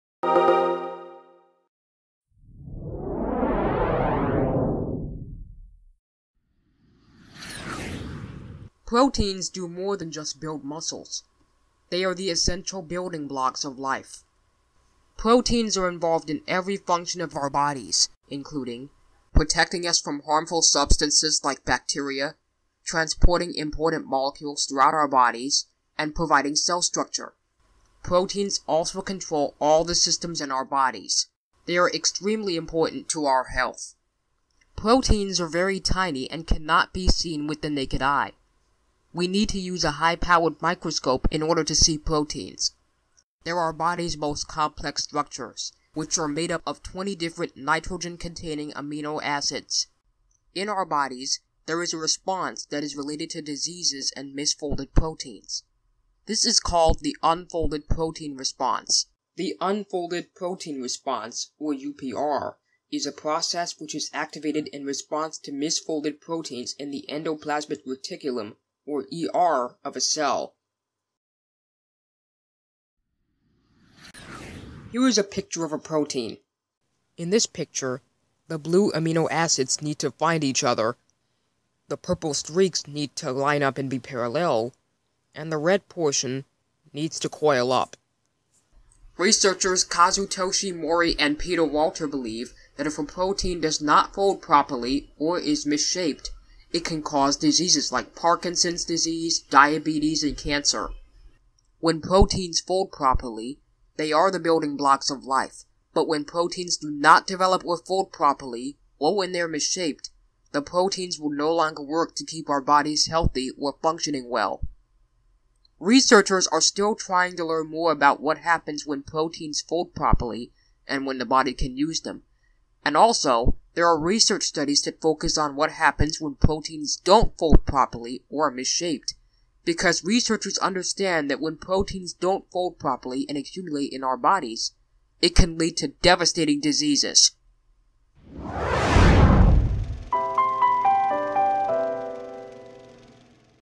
breakthroughjuniorchallenge: Unfolded Protein Response (Life Sciences)
In our bodies, there is a response that is related to diseases and misfolded proteins. This is called the unfolded protein response, or UPR. This is a video I did for the Breakthrough Junior Challenge on the unfolded protein response.